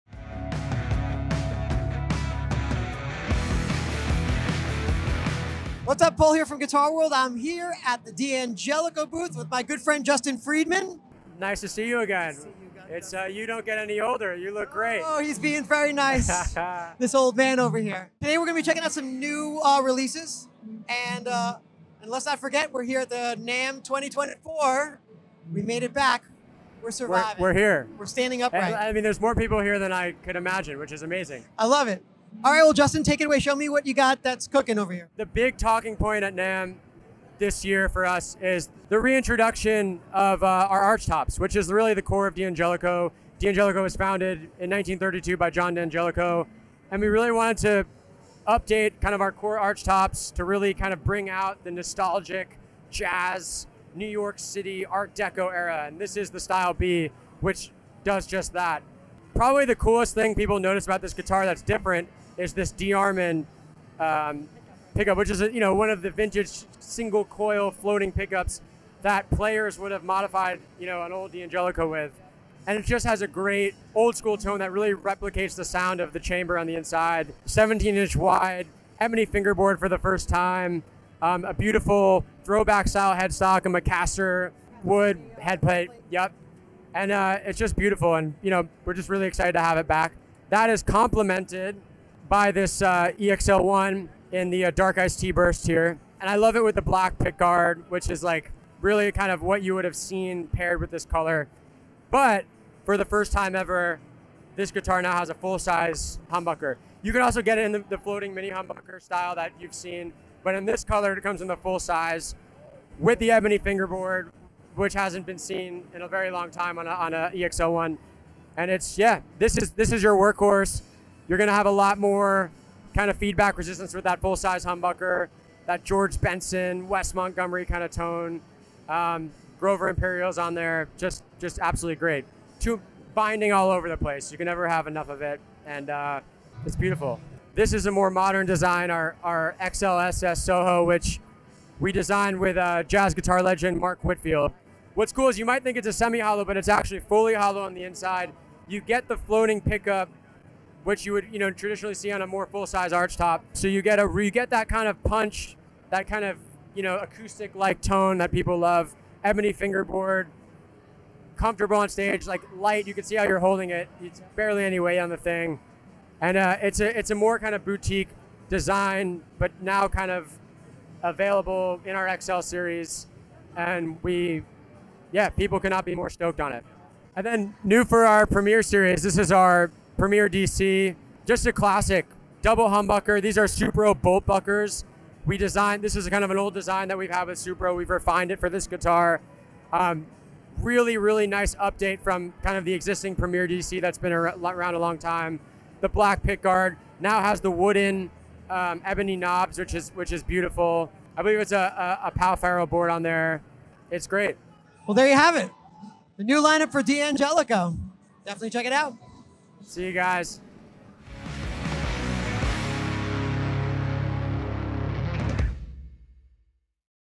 NAMM 2024: D’Angelico’s latest lineup of luxury electric guitars
Guitar World’s Tech Editor, Paul Riario, ventured to the D’Angelico NAMM booth to get the low-down on the brand’s latest luxury releases.